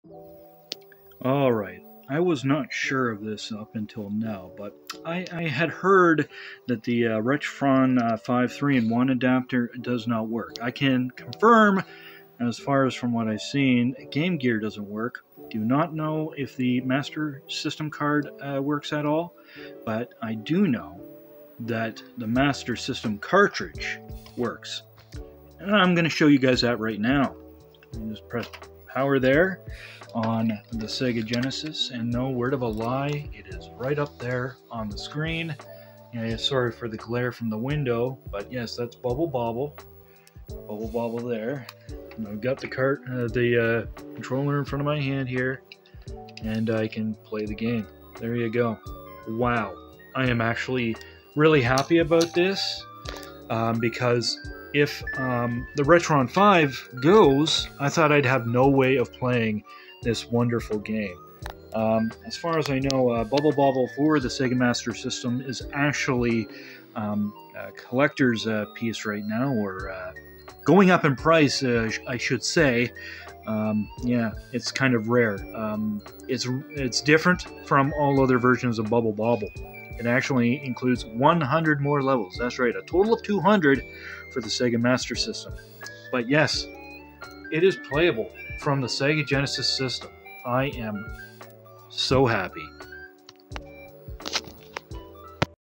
Master System is Playable on Genesis via the Retron 5, 3 in 1 adapter.
I wasn't aware of this until now but Master System is playable on the Genesis via the Retron 5 3 in 1 cart adapter.